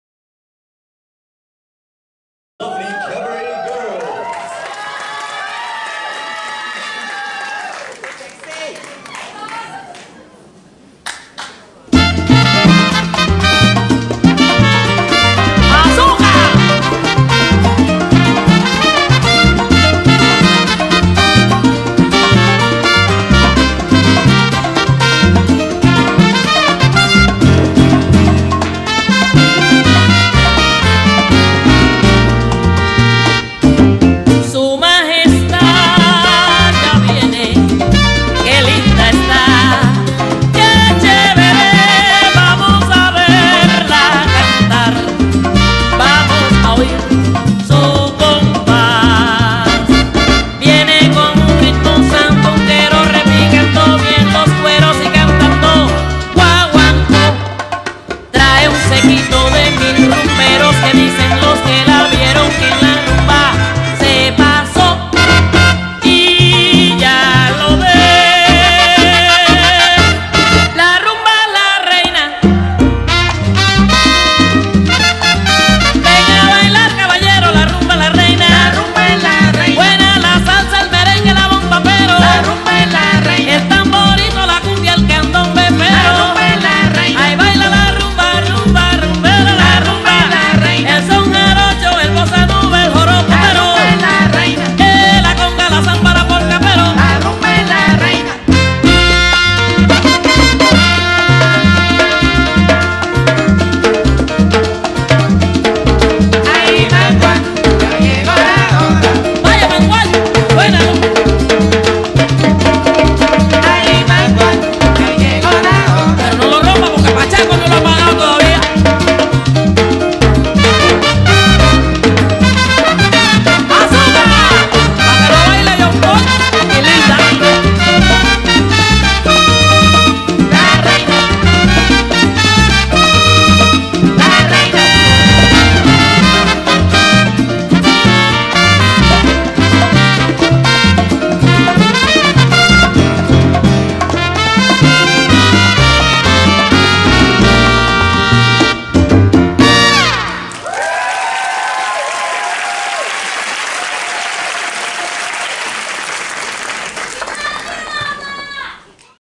Alumni Ladies’ Shines Team – Reina Rumba at SMU Caderas Latinas’ Bailamos 2017
Showcased at 2017 Bailamos XIII: The Golden Ticket
Performed by SMU Caderas Latinas Alumni Ladies’ Shines Team
Choreographed by SMU Caderas Latinas Alumni Ladies’ Shines Team